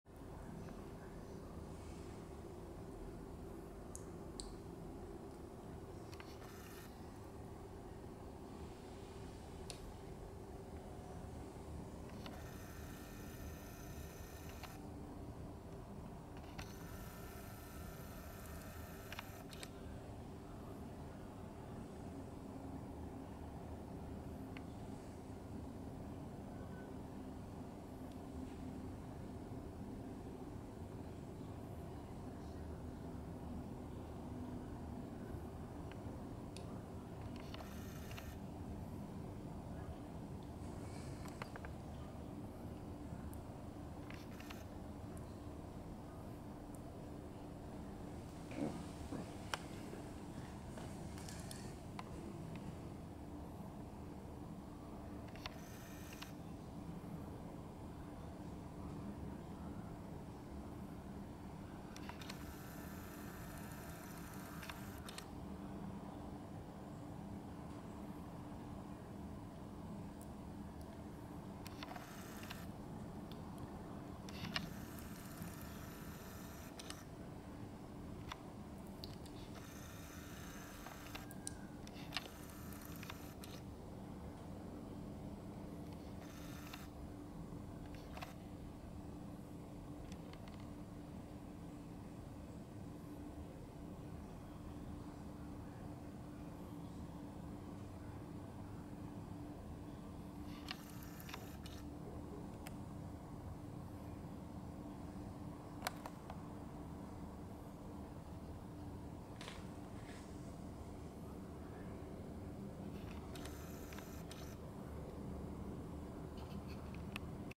2 girl badgers out for dinner!
2 girl badgers having dinner.